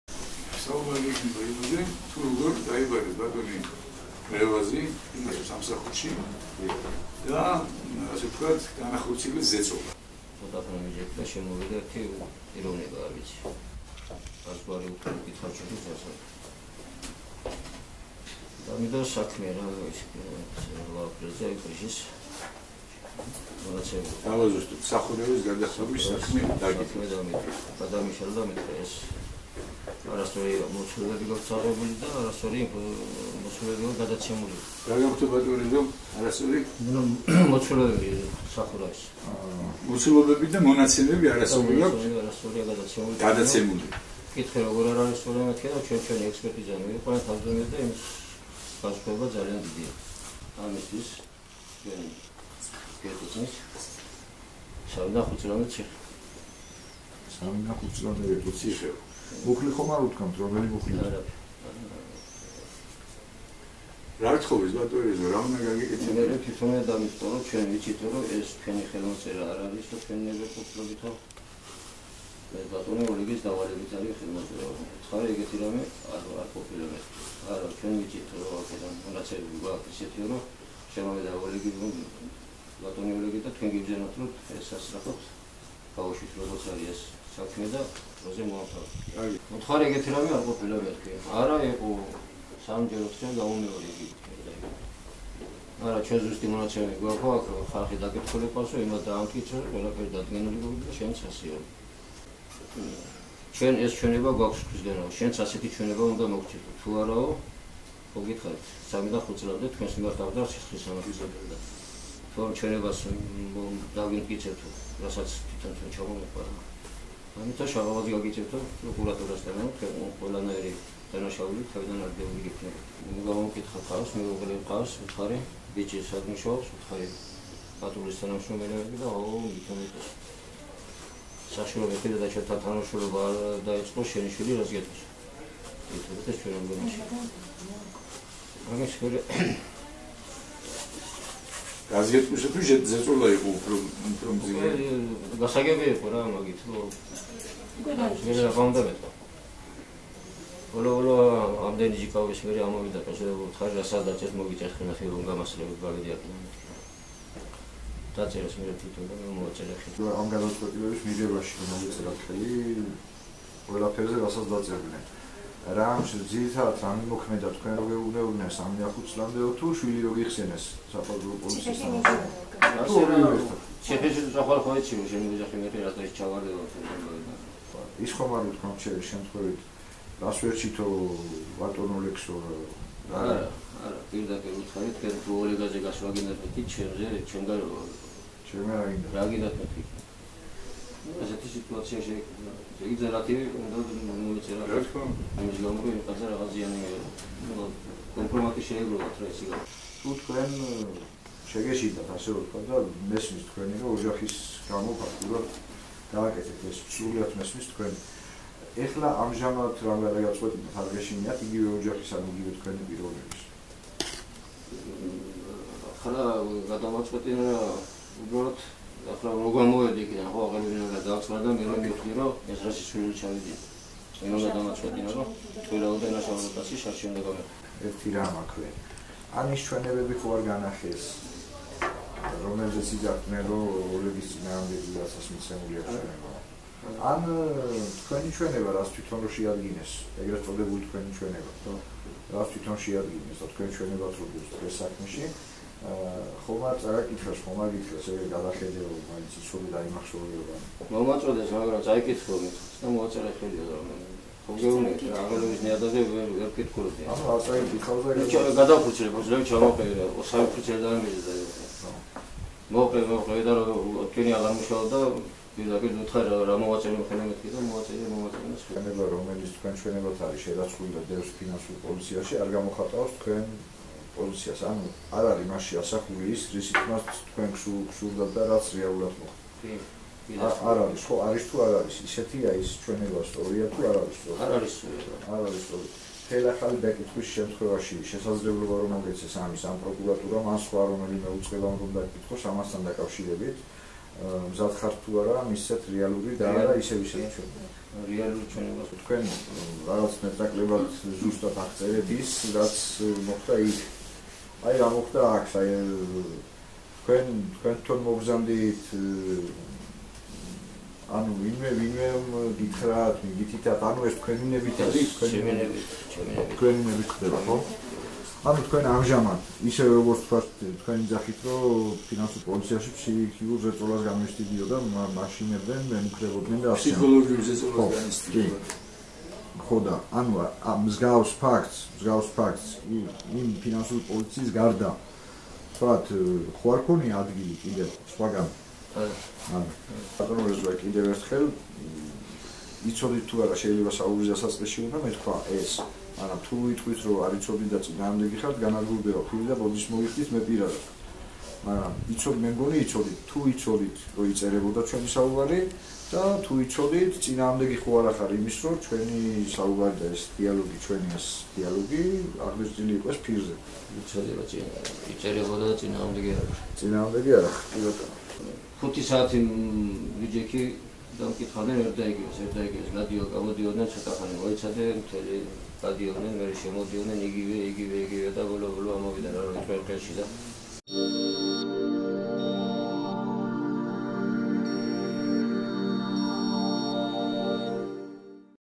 რევაზ მერაბიშვილი დაკითხვის დეტალებს იხსენებს
ვიდეომასალაში, რომელიც ახალციხის მუნიციპალიტეტის საკრებულოს შენობაშია ჩაწერილი, გამგეობის თანამშრომელი რევაზ მერაბიშვილი იხსენებს, ფინანსთა სამინისტროს საგამოძიებო სამსახურის სამცხე-ჯავახეთის სამმართველოში, 27 მარტს, მისი დაკითხვის დეტალებს.